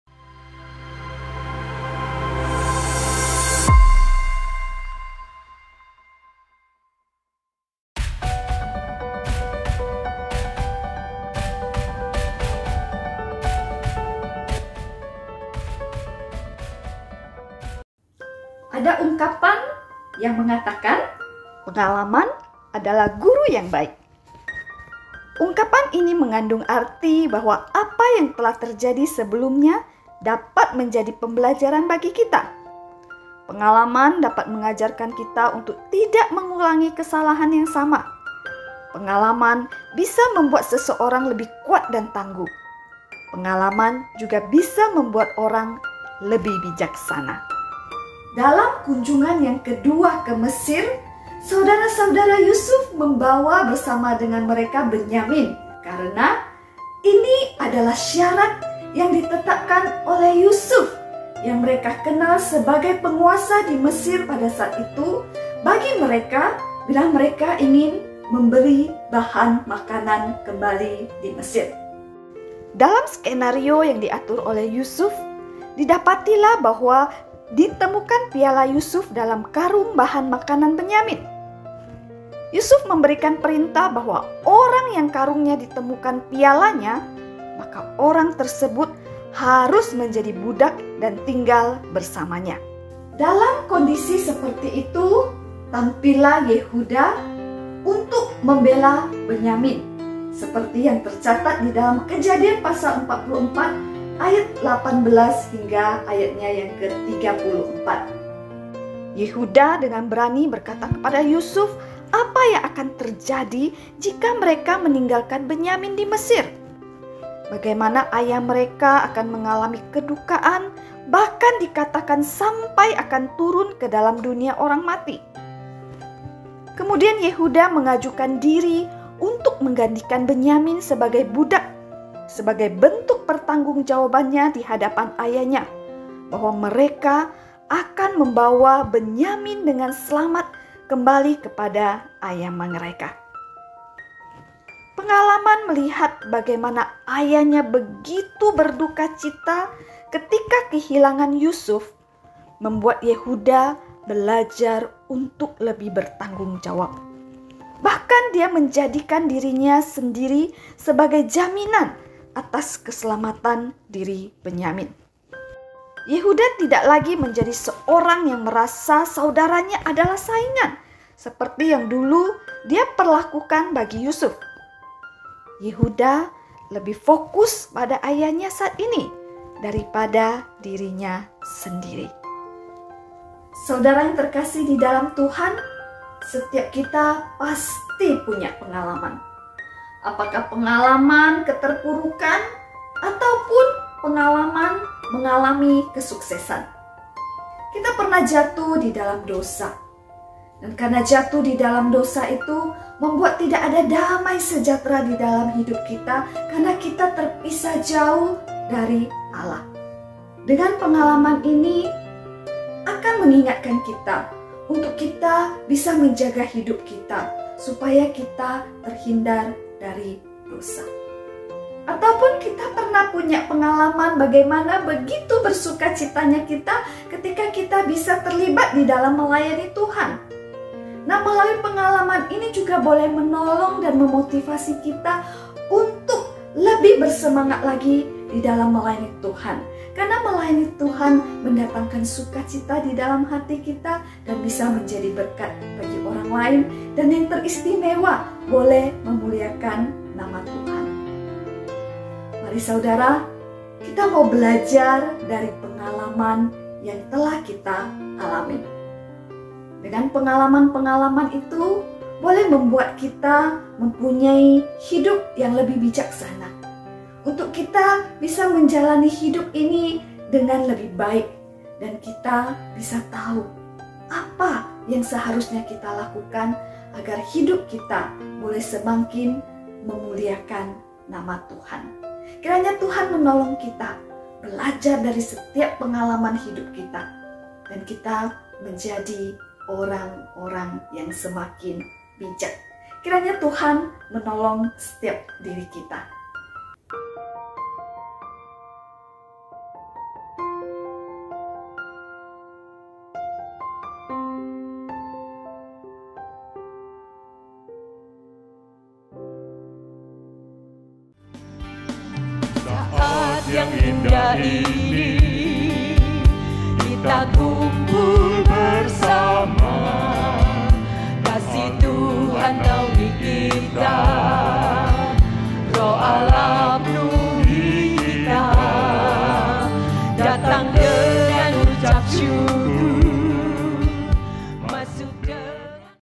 Ev.Santi Hendriani - Belajar dari Pengalaman
Renungan harian GKKB Jemaat Pontianak
Seri Kitab Kejadian - Belajar dari Pengalaman  (Kejadian 44:18-34)